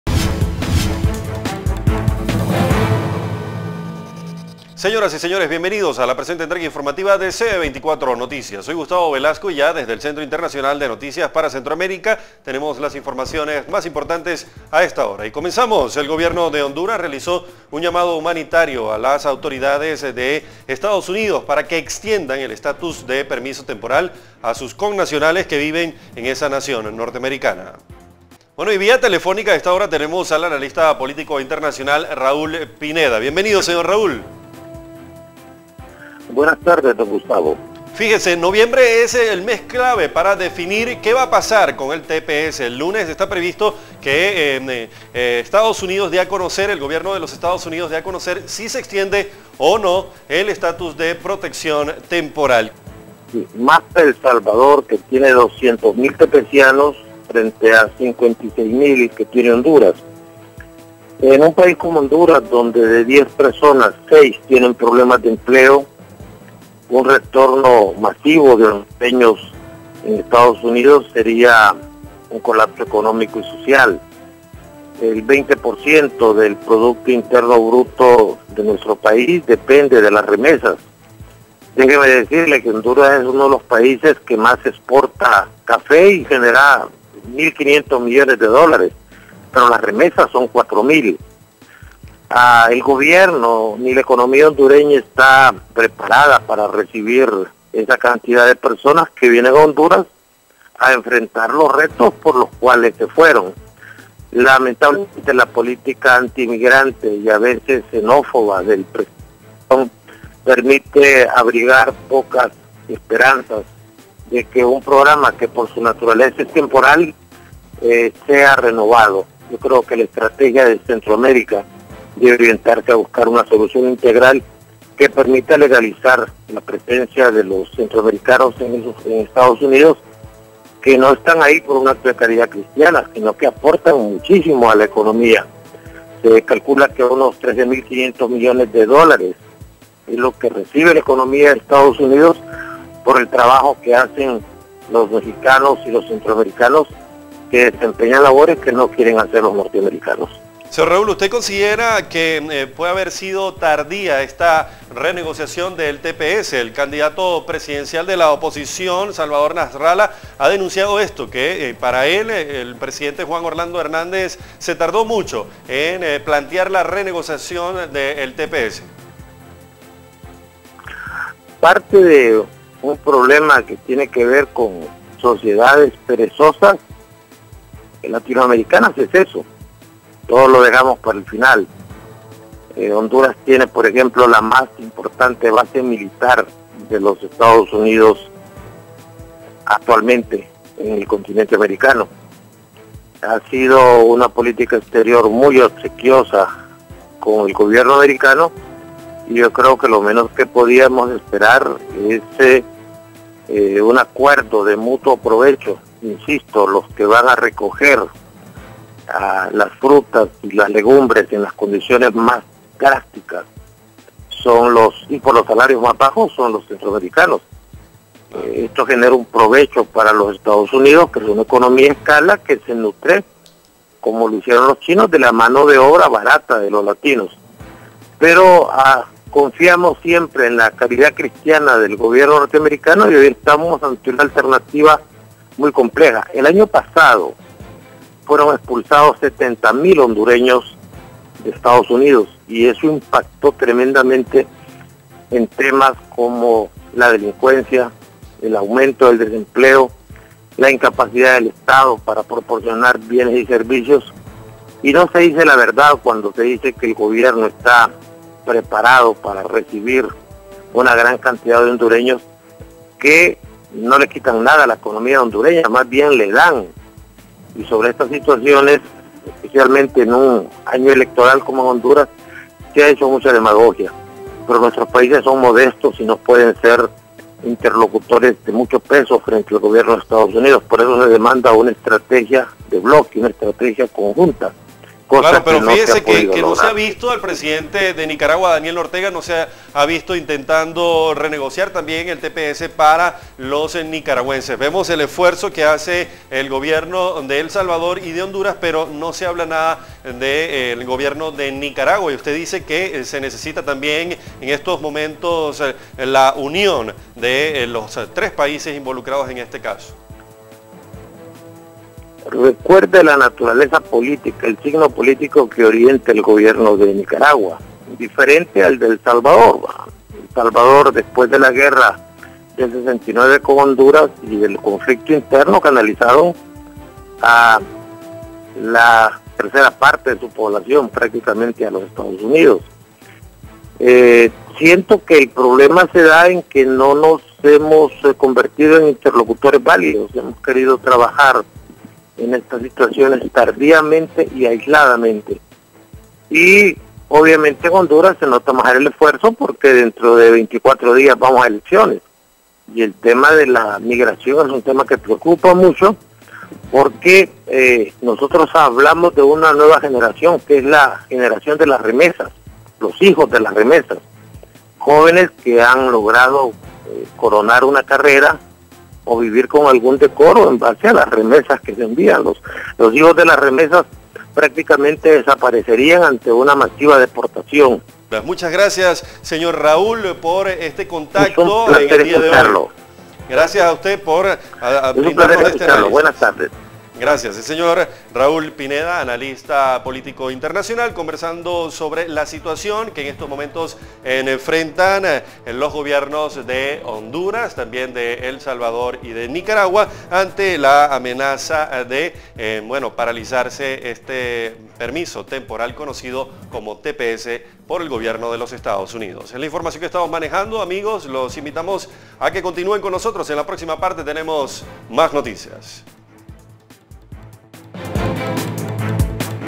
Fin de TPS pondría en peligro economías centroamericanas
El temor ante la posible  eliminación del permiso temporal conocido como TPS tiene en vilo a hondureños, salvadoreños y nicaragüenses,  beneficiarios de este amparo migratorio por parte de los Estados Unidos a ciudadanos de estas naciones que abandonaron sus familias para irse a alcanzar el sueño americano.

A pocas horas de conocerse qué decisión podría tomar el gobierno del presidente Donald Trump sobre el TPS,   CB24 noticias de Centroamérica en exclusiva, vía telefónica, entrevistó al analista político internacional, Raúl Pineda.

Para el analista “el peor panorama es para las economías de El Salvador y de Honduras que  se nutren mayoritariamente de las remesas, que envían los beneficiarios del TPS desde los Estados Unidos”.

Pineda advierte que los gobiernos de Honduras y El Salvador, no han creado las condiciones en sus países para poder recibir y darle respuesta a miles de beneficiarios del TPS que serían deportados si se acaba el beneficio.

El experto indica que se reconoce el esfuerzo que están haciendo los gobiernos de El Salvador y Honduras, moviendo su artillería para renegociar el TPS, sin embargo mientras se termina de definir el futuro del permiso, los beneficiarios tienen la esperanza de que se defina su situación y aspiran que se legalice su estatus.